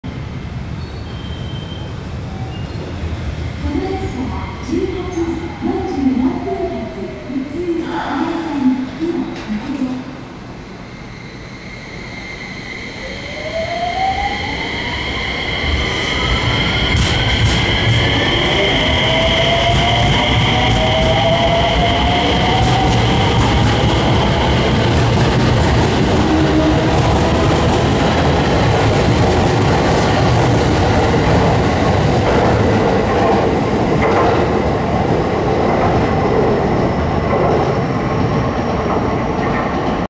【4K】JR山陽本線 普通列車227系電車 ﾋﾛA63編成+ﾋﾛA56編成 三原駅発車
広島地区を中心に活躍している新型車両です